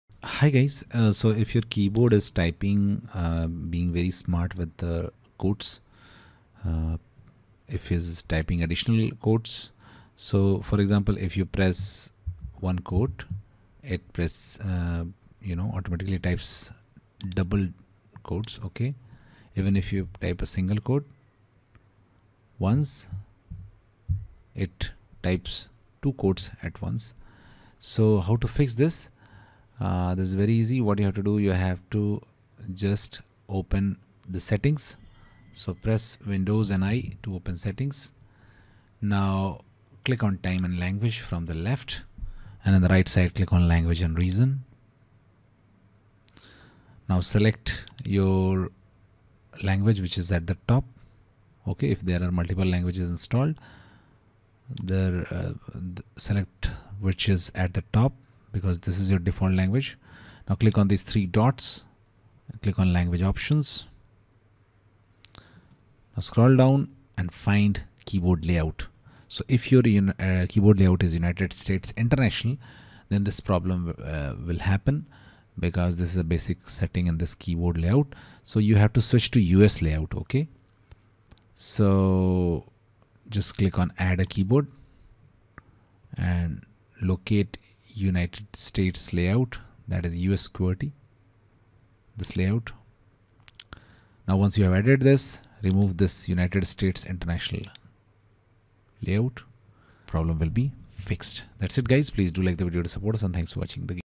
How to Fix extra Double quotes keyboard issue Windows 11
How to Fix Double quotes keyboard issue Windows 11

Additional double quotes problem in windows 11